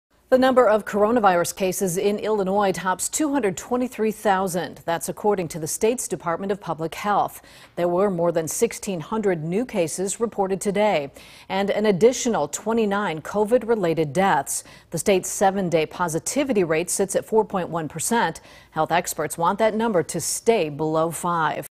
Illinois reports 1,680 new cases of COVID-19 Tuesday, 5 deaths in Winnebago County
The Illinois Department of Public Health today announced 1,680 new confirmed cases of coronavirus  in Illinois, including 29 additional confirmed deaths.